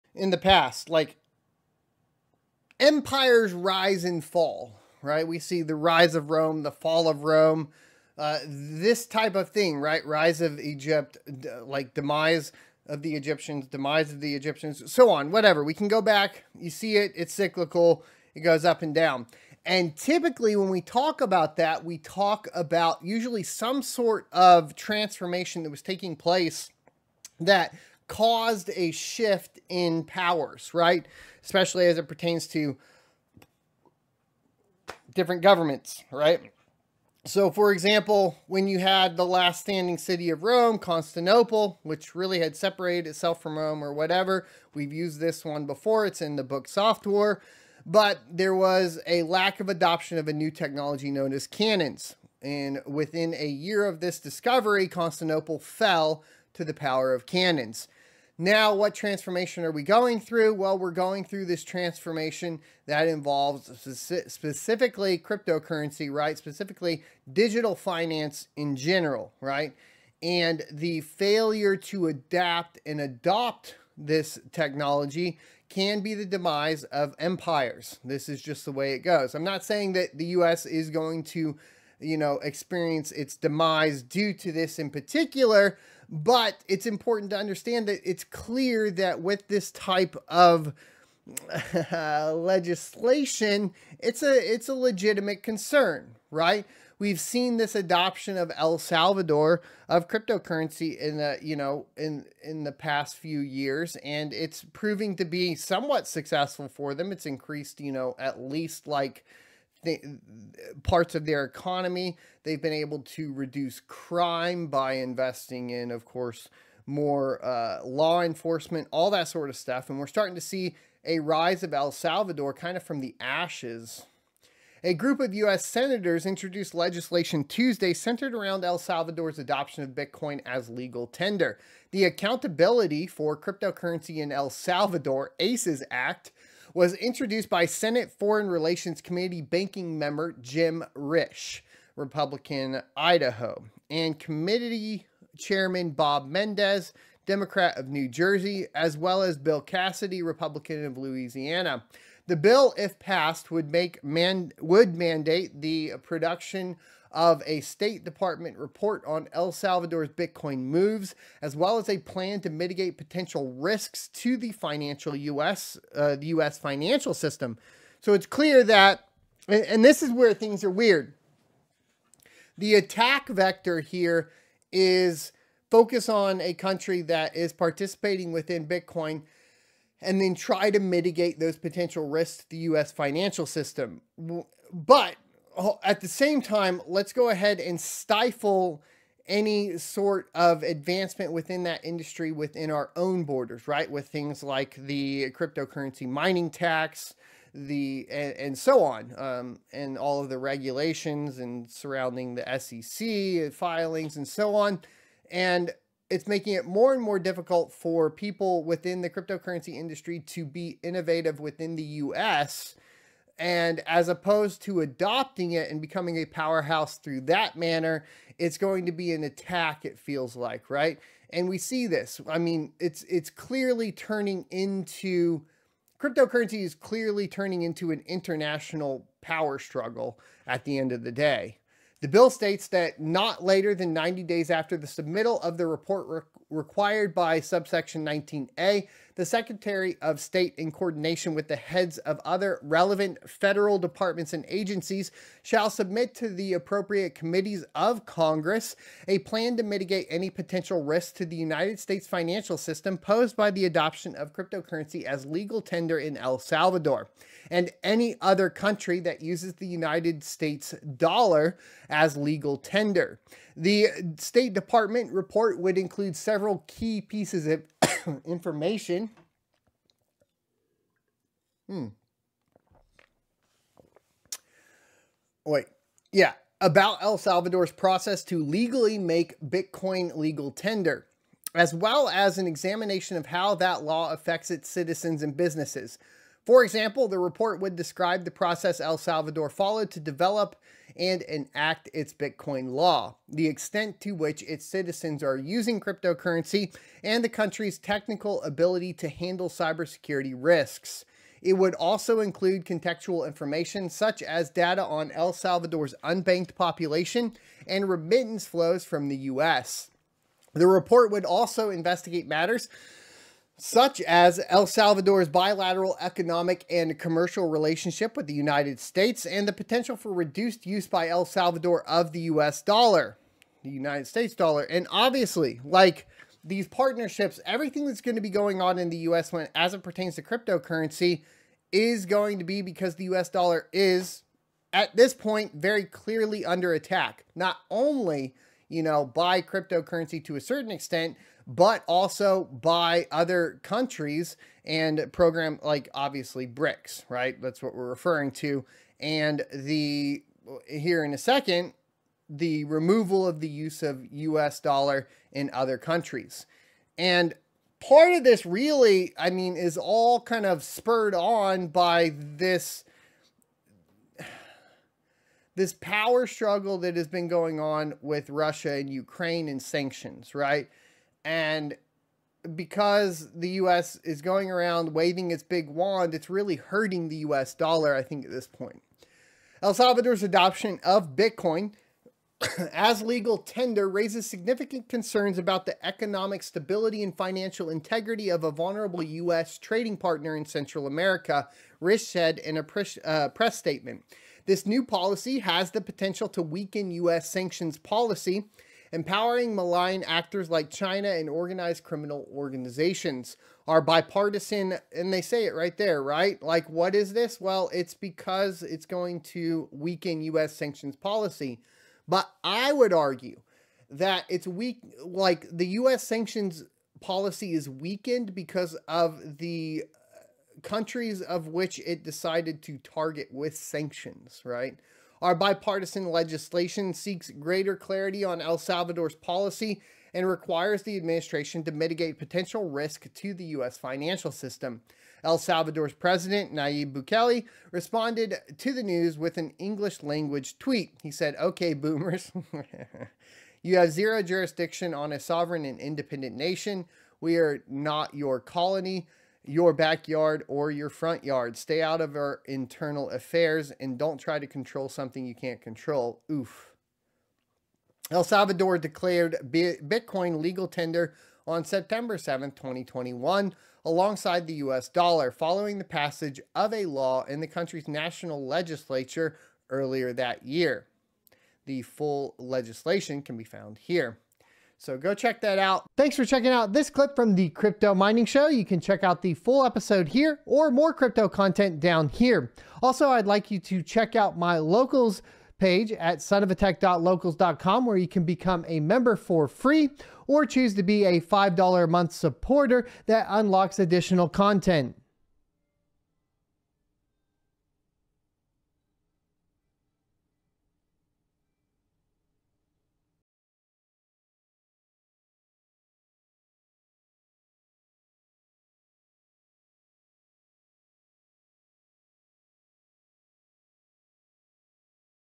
Bitcoin Goes Political: Senators Demand State Department Report on El Salvador's Crypto Adoption-253
Crypto Mining Ecourse is Live!

New US Senate legislation mandates plan to 'mitigate potential risks' of El Salvador's bitcoin adoption

Ergo Address: 9fCyhudURbDSA2qi9k3sh1LgbHwyFwT6fck7wQW2YYWk9xR1miN
ETH Address: 0x8446b70aA05c811d7bF590e6EDE3d90C00391FcE
ETC Address: 0x8446b70aA05c811d7bF590e6EDE3d90C00391FcE
ZIL Address: zil135ajqdgf9mj6r50e8t28p98xvsss8ay7cppyx5